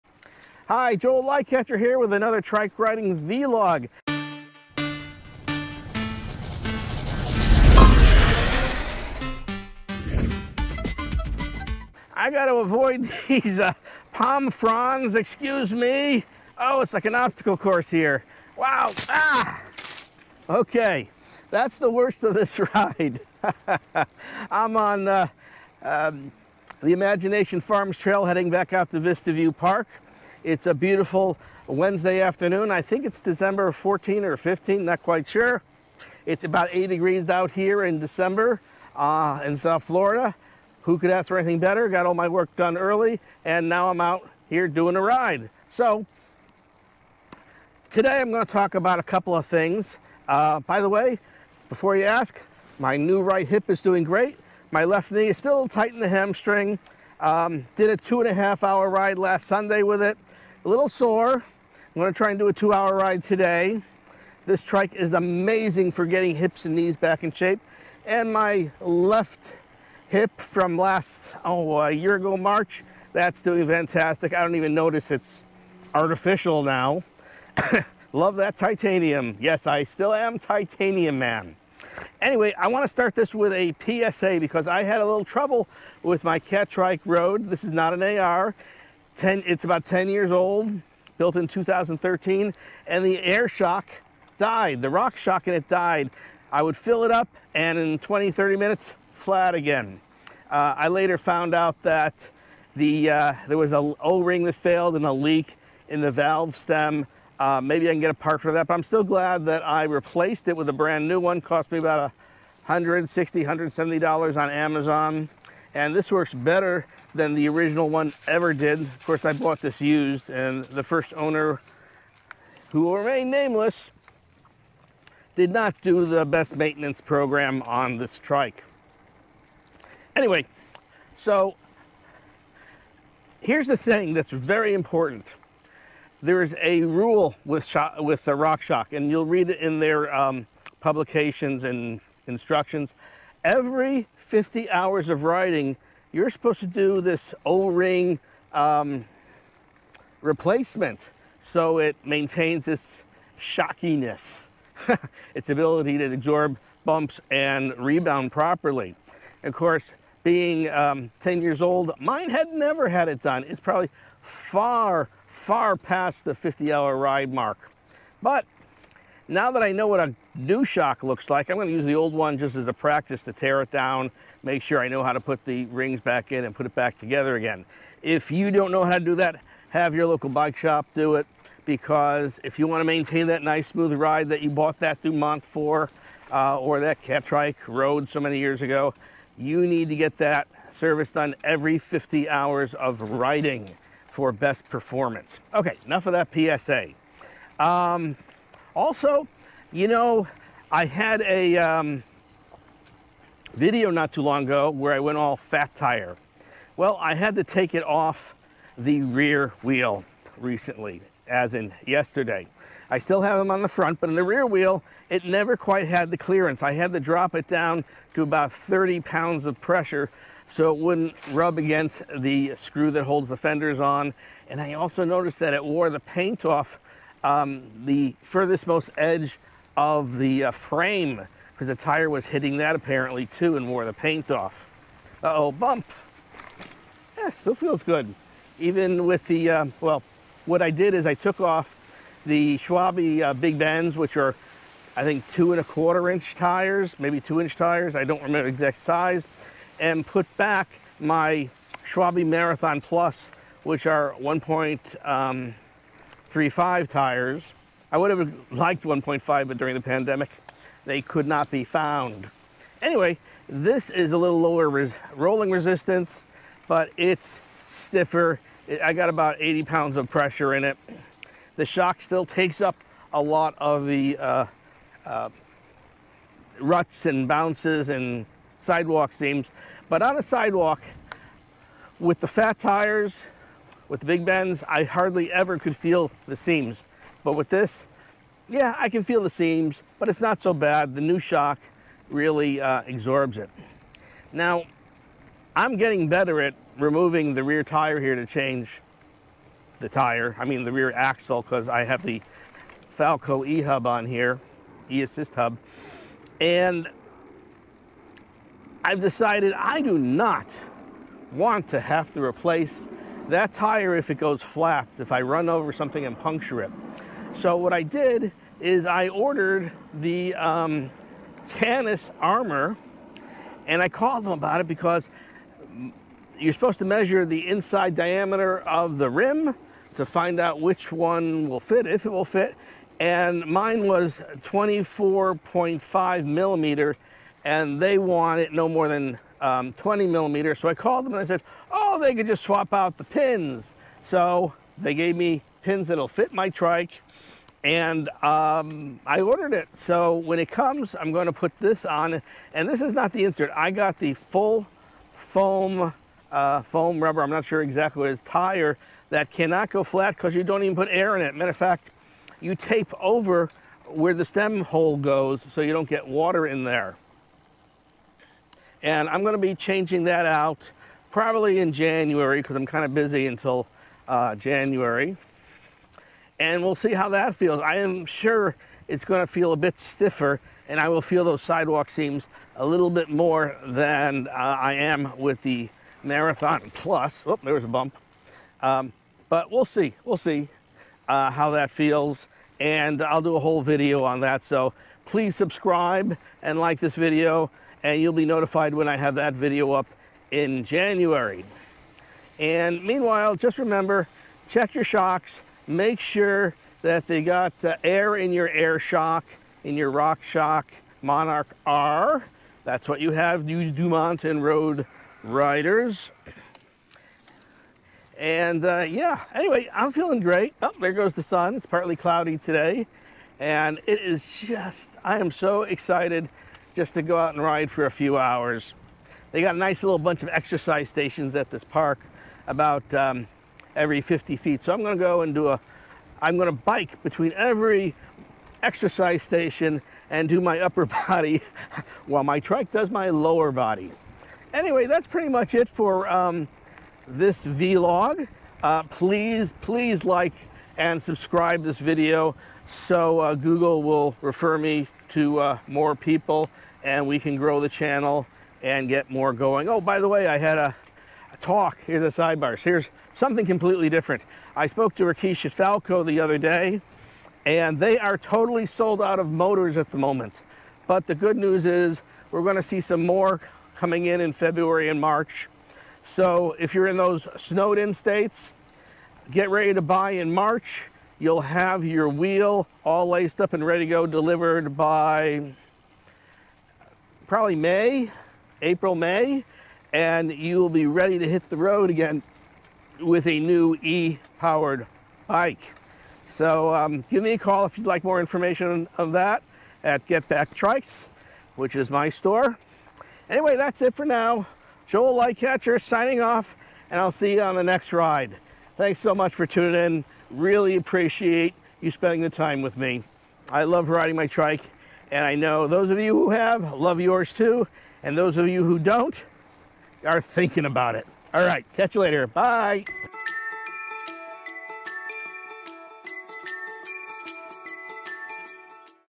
vLog December 14, 2022
Talking about:
1) Catrike Dumont & Road Airshox service.
2)Trike Tires: Schwabe BigBen Plus vs. Marathon Plus vs. Tannus solid tire.
3) Falco Hub Motors in stock very soon.